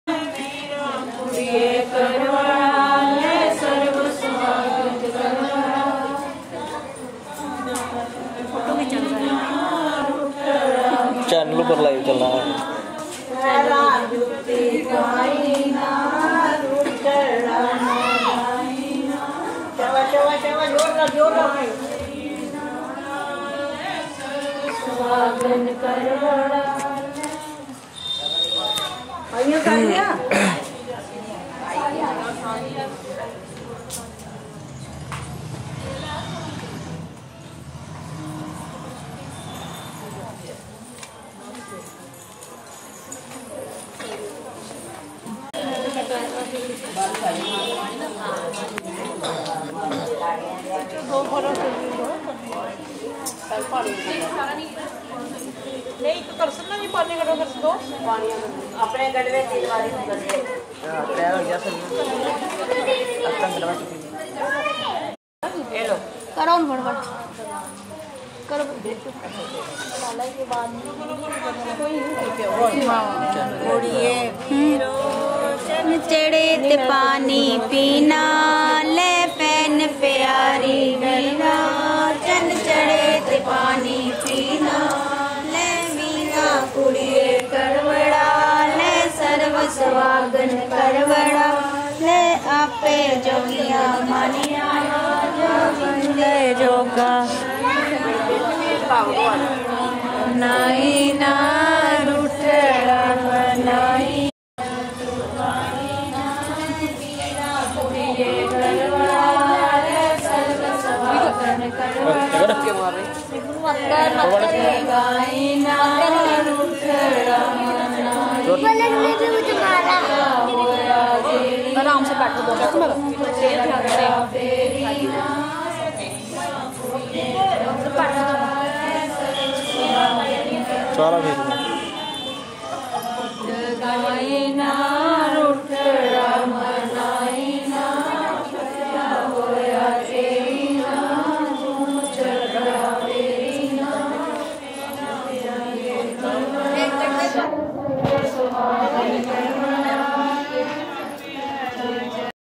करवा चौथ बख्शी नगर सुहागिनों के पवित्र तोहार करवा चौथ के उपलक्ष पर आज शाम.
करवा चौथ बख्शी नगर सुहागिनों के पवित्र तोहार करवा चौथ के उपलक्ष पर आज शाम को ही लोगों ने मंदिरों और अपने घर के आंगन में सुहागिनों के साथ पूजा अर्चना की और साथ में कर् देवी की पूजा भी की.